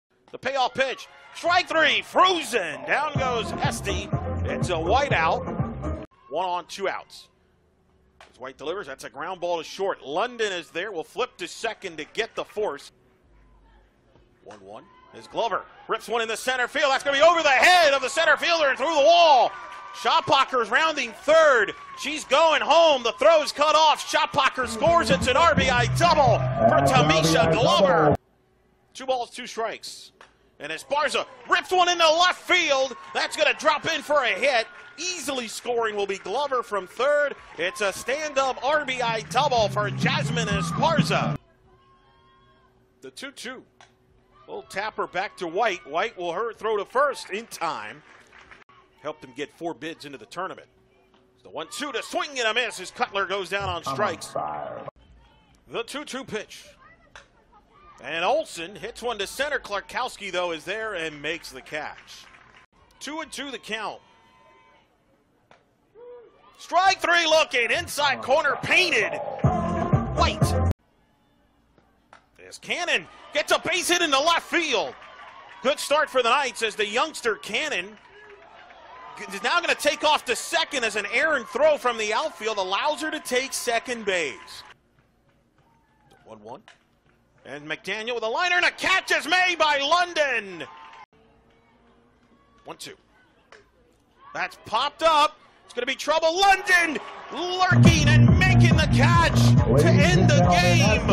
Highlights: UCF SB vs UConn Game 1
Watch some of the top plays from UCF's 2-0 win over UConn on Friday, March 22 to start American Athletic Conference play.